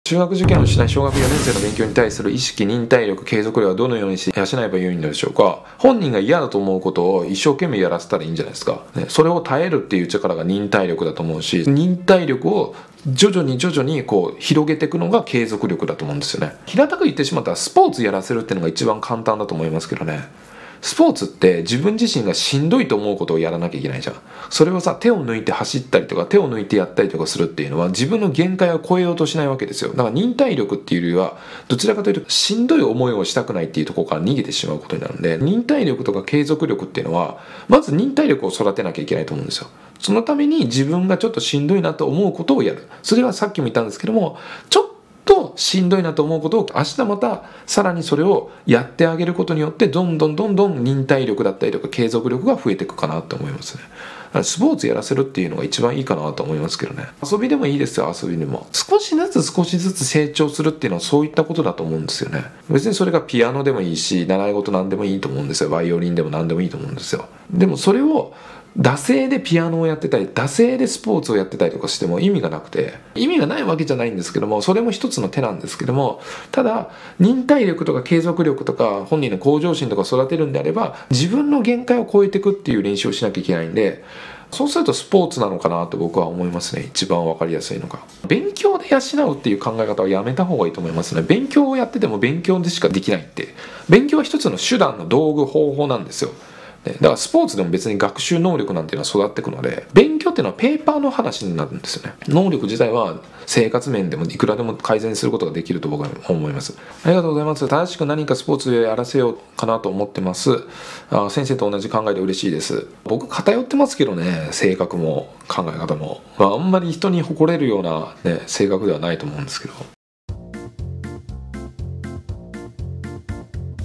【塾講師の教育相談LIVE】中学受験をしない小学4年生の勉強に対する意識、忍耐力、継続力はどのように養えばいいのでしょうか？【LIVE切り抜き】
問題集に万能薬はありません

○リンク